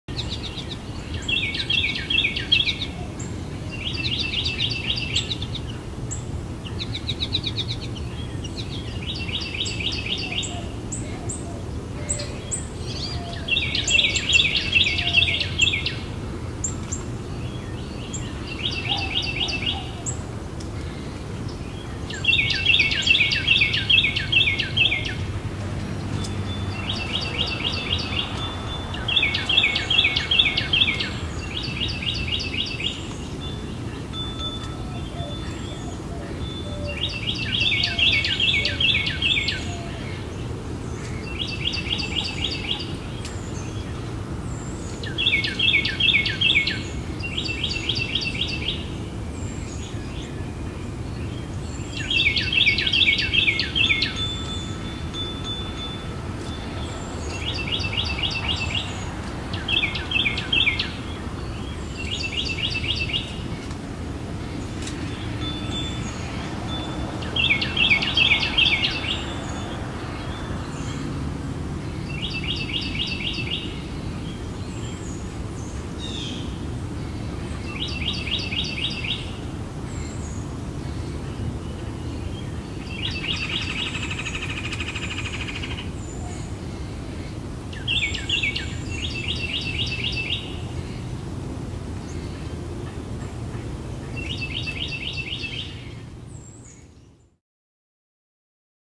Monarch Caterpillars Eating FYV
Monarch Caterpillars (Danaus plexippus) eating milkweed.  Fifth instar larva. Over 2 dozen caterpillars have been eating here recently. Many have already entered the pupa or chrysalis stage now. UHD 4K FrontYardVideo FYV